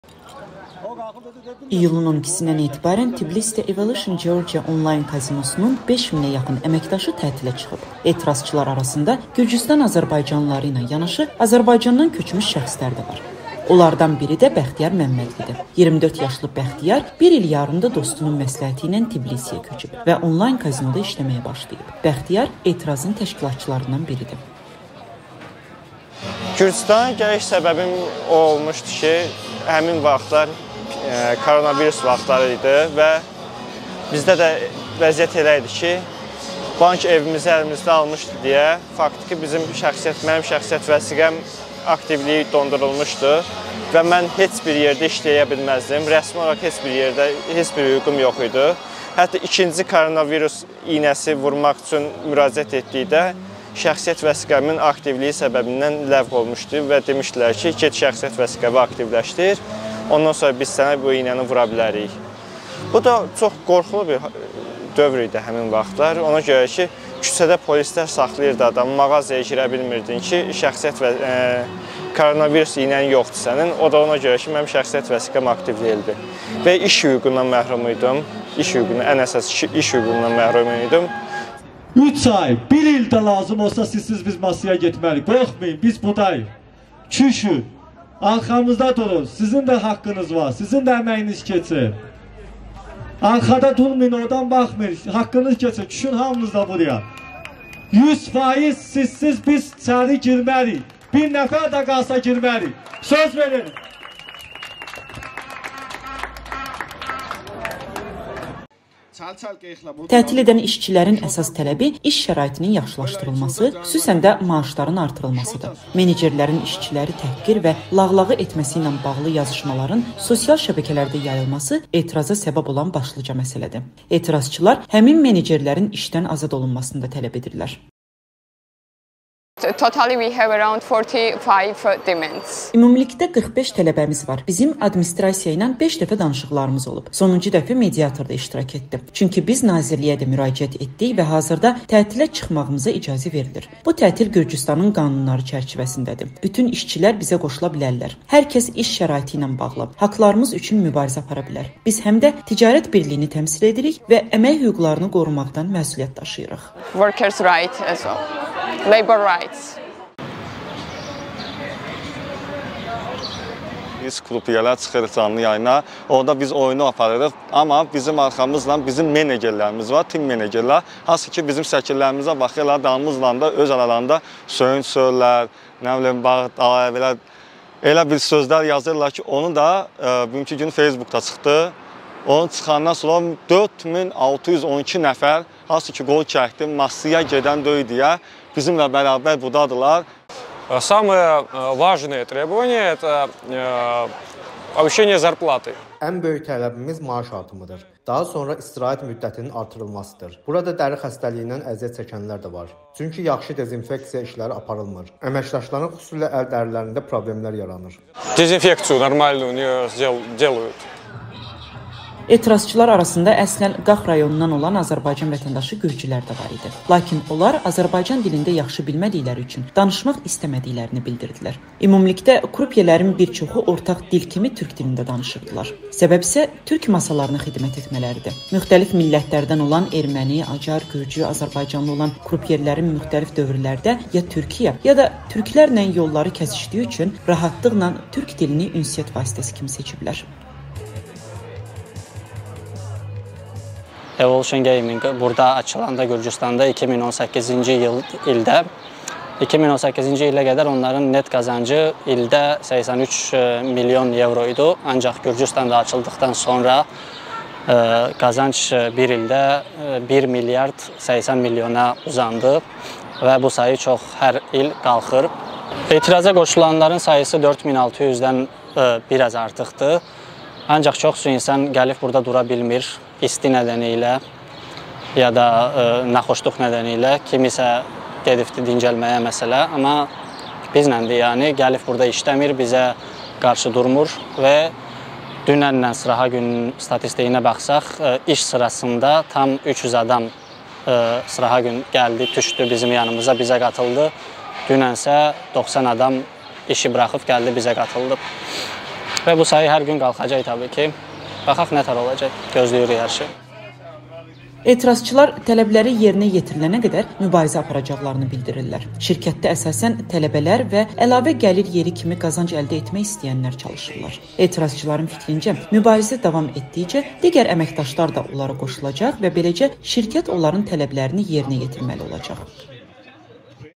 Tbilisidə “Evolution Georgia” onlayn kazinosunun 5000-ə yaxın əməkdaşı tətilə çıxıb
İyulun 12-dən etibarən Tbilisidə “Evolution Georgia” onlayn kazinosunun 5000-ə yaxın əməkdaşı tətilə çıxıb. Etirazçılar arasında Gürcüstan azərbaycanlıları ilə yanaşı Azərbaycandan köçmüş şəxslər də var. Onlardan biri də Bəxtiyar Məmmədlidir. 24 yaşlı Bəxtiyar bir ilyarımdır dostunun məsləhəti ilə Tbilisiyə köçüb və onlayn kazinoda işləməyə başlayıb.